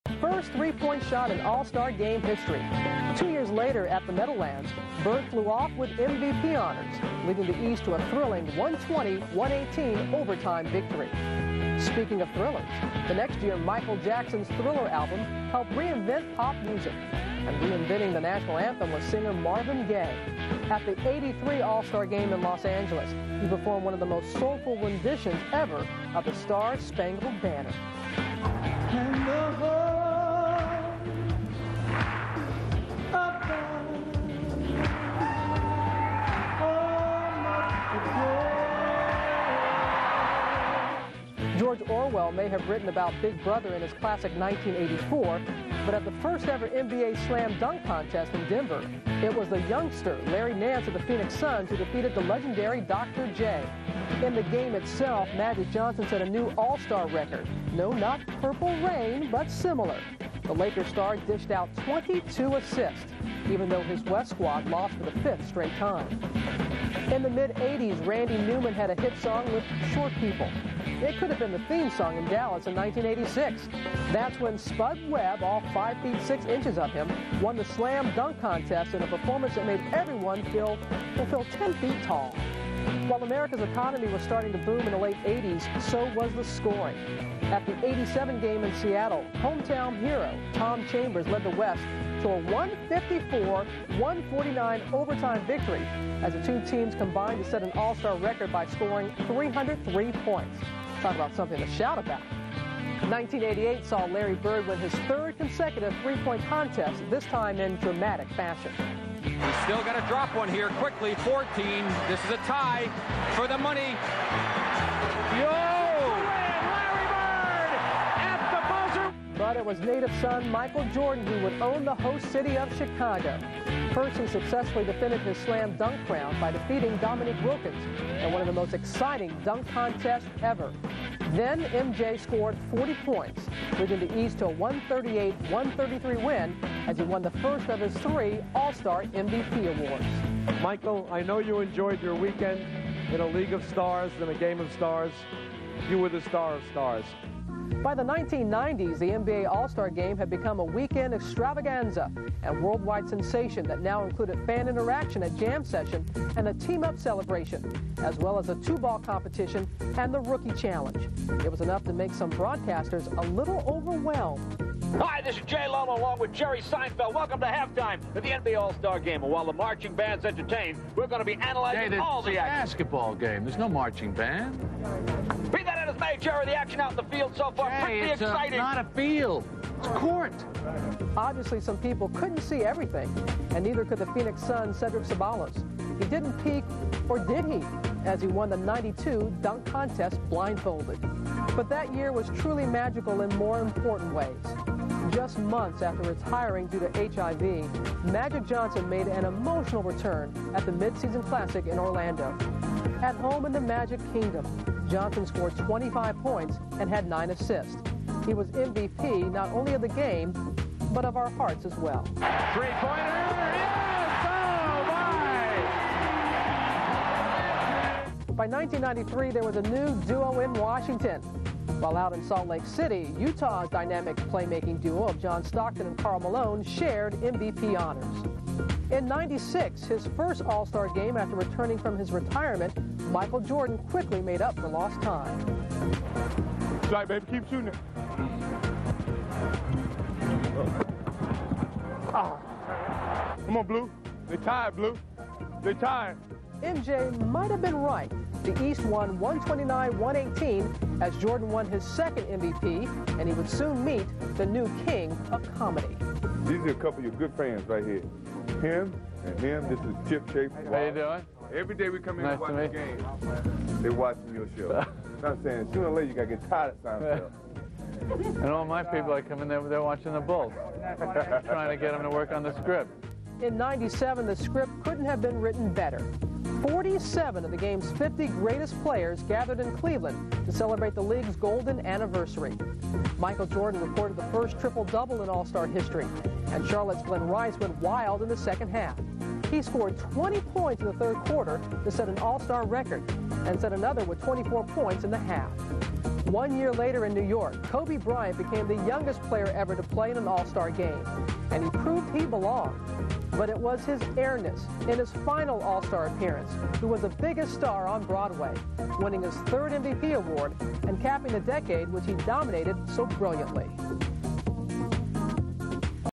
History of the NBA All Star Game 3/3
History of the NBA All Star Game

Highlights from Larry Bird, Larry Nance, Earvin "Magic" Johnson, Spud Webb, Tom Chambers, Cedric Ceballos, Stockton and Malone, and His Airness Michael Jordan